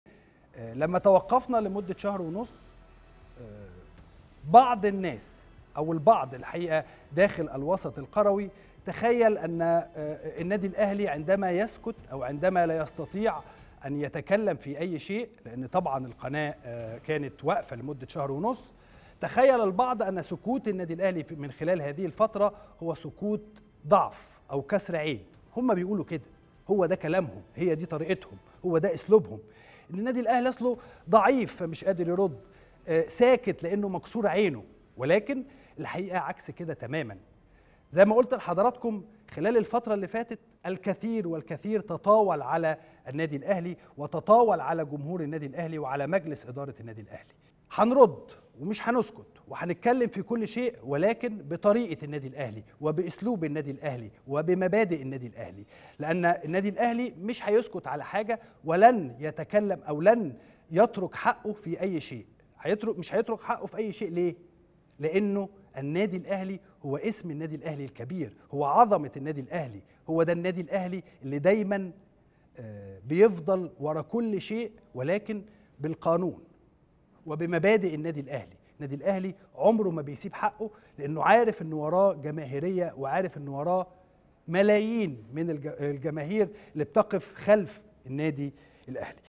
اسلام الشاطر: الاهلي اسم كبير.. والبعض تخيل ان سكوته ضعف
اسلام الشاطر: الاهلي اسم كبير.. والبعض تخيل ان سكوته ضغف

التردد: 12341 أفقي.
القمر: نايل سات.
معامل الخطأ: 5/6.
معدل الترميز: 27500